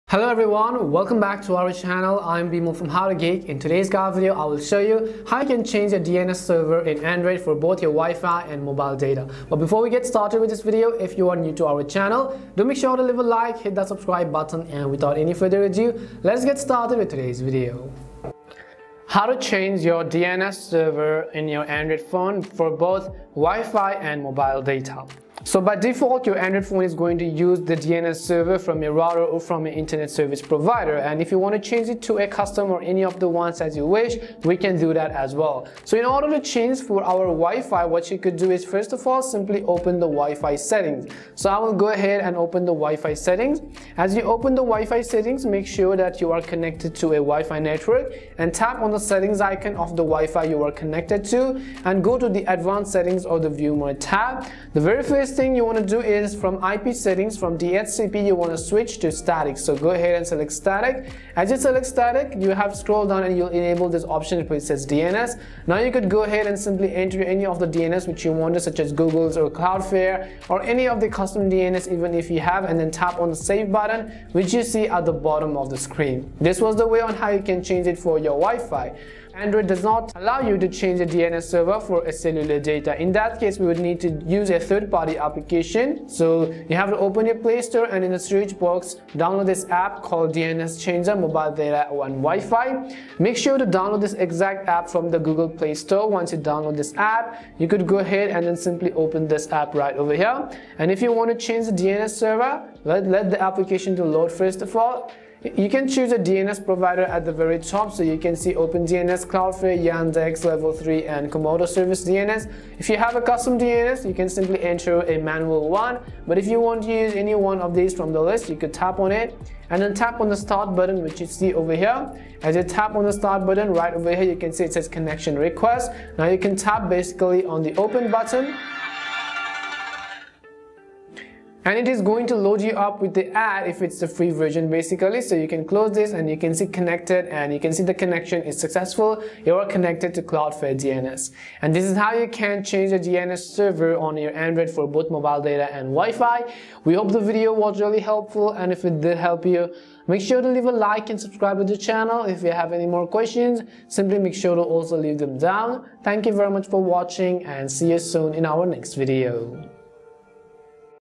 Change DNS Server in Android [for Mobile Data & WiFi]
In this tutorial video, I will quickly guide you on how you can change your DNS server in Android for both Mobile Data as well as WiFi.

1) Open WiFi Settings
2) Change IP settings to Static
3) Now you can go ahead and enter your custom DNS
4) For Cellular Data, download an app called DNS Server
5) Open the App and choose your DNS server
6) Enter custom if you want.
7) Tap on Start at the bottom.
8) This way, you can change your DNS server on Android.

0:00 Introduction
0:18 Change DNS Server for WiFi
1:19 Change DNS Server for Mobile Data
2:21 Outro: Final Verdict